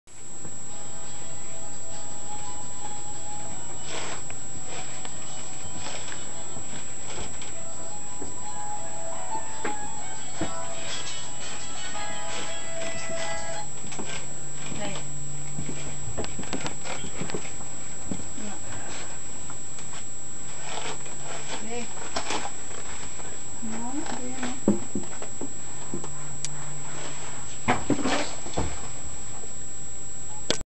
M3353Monaco40DFT2008
2008 Monaco Knight 40DFT 40' Class A Motor Coach -- 3 slides, patio awning, 15.0 ducted A/C, 3 camera rear vision system, leather love seat, sofa bed, 5 disc changer DVD player, 2 TV's, table w/2 chairs, fully equipped kitchen, stainless steel package, double door refrigerator, ceiling fan, walk around queen bed, 1.5 bath, private full bath in bedroom.  PLEASE CALL FOR SPECIAL INTERNET PRICE.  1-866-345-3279